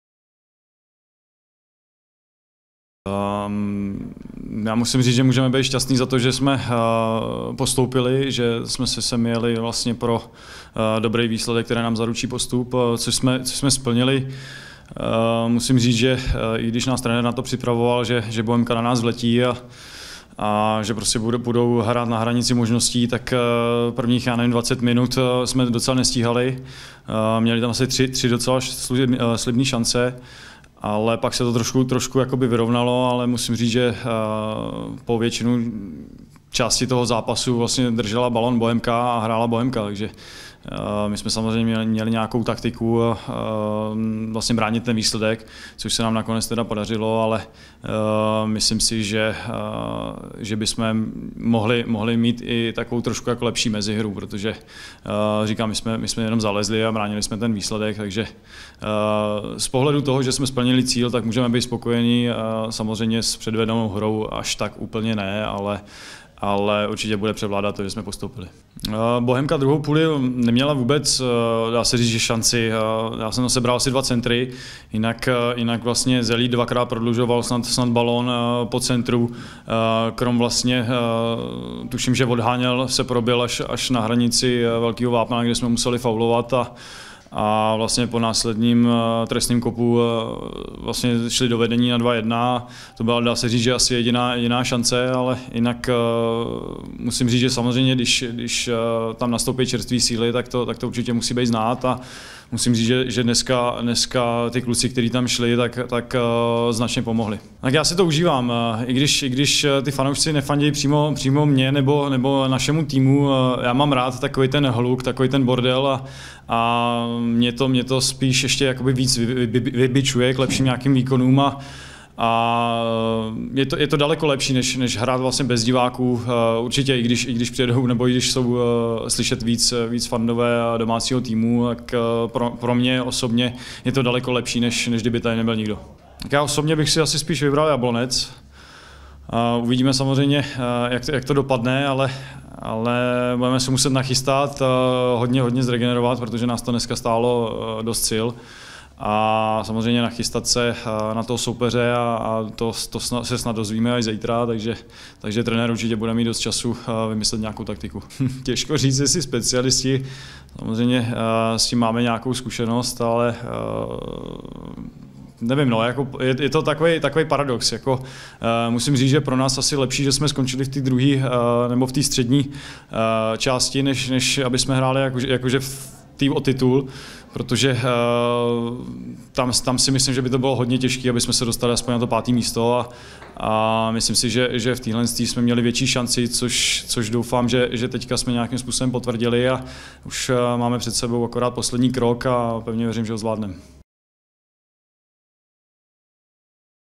Jan Šeda po utkání Bohemians Boleslav
Hodnocení utkání od Jana Šedy, tlak Bohemians s fanoušky za zádech, příchod zkušených hráčů ve druhém poločase a o možném soupeři z pátého místa pro BolkaTV.